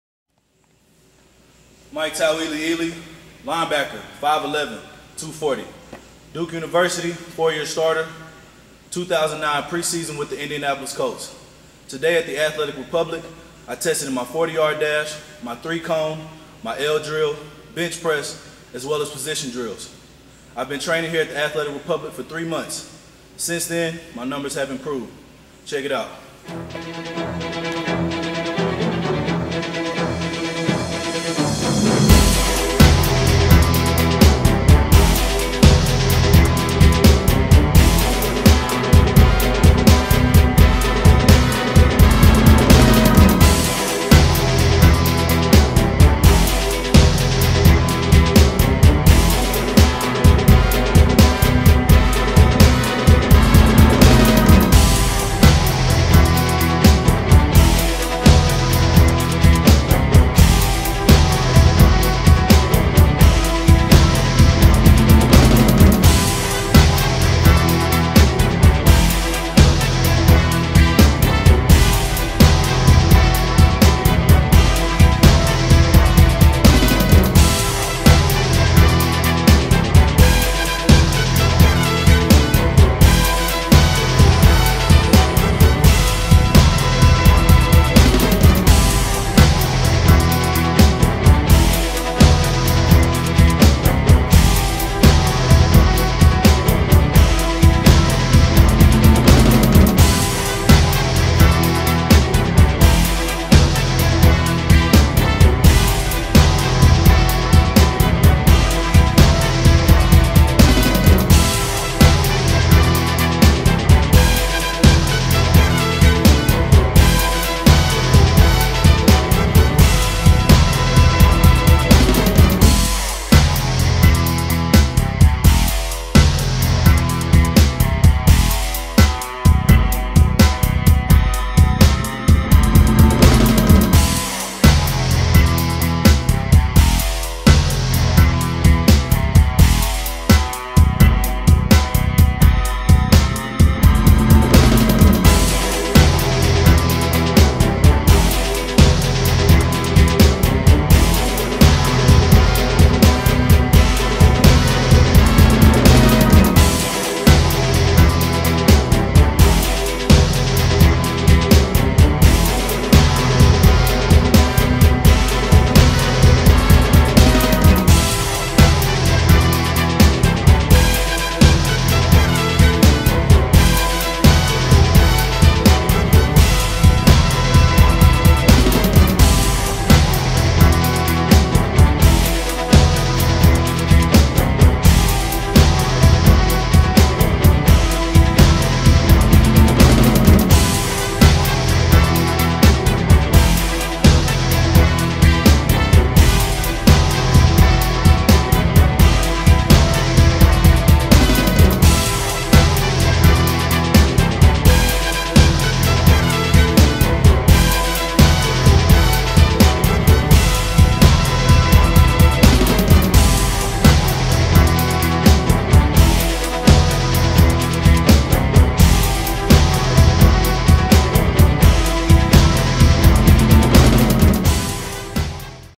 Mike Tauiliili combine workout video
Mike Tauiliili test numbers in 40 yard dash, 3 cone drill, L drill, 225 lbs reps, and position work on the field.  Mike Tauiliiili was tested at Athletic Republic in Houston, Texas on April 30, 2010.

Test Numbers:
40 yd dash: 4.71 & 4.68
3 cone: 4.12 & 4.09
L drill: 6.80 & 6.69
225 lb: 22

Mike Tauiliili was in the 2009 training camp with the Indianapolis Colts  and played in all four pre-season games.  Since being released he has been training daily at Athletic Republic located in Houston, Texas

Mike Tauiliili played at Duke University as a 4 year starter where he earned All American and All ACC honors during his senior year campaign.  Mike was invited to the East-West Shriners Senior All-Star game and was elected team captain by his coaches and peers.  Mike went on to tally 18 tackles and was named Defensive Player of the Game.